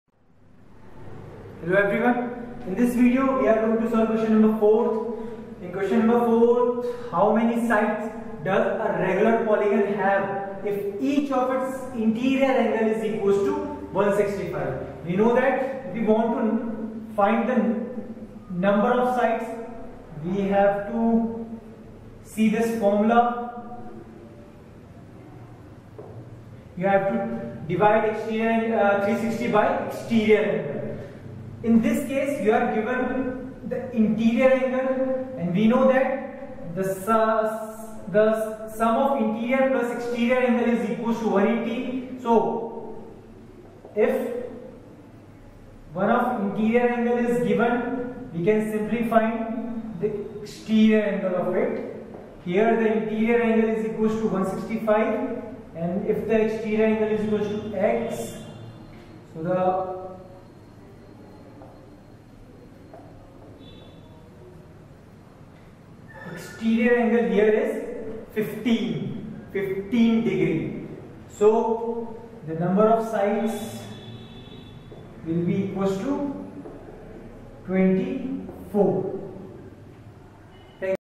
Exercise 3.2 - Question 4 NCERT/CBSE Solutions for Class 8th Maths (Understanding Quadrilaterals)
In this video we are solving class 8th NCERT - Chapter 3 Understanding Quadrilaterals Exercise 3.2(Question-4).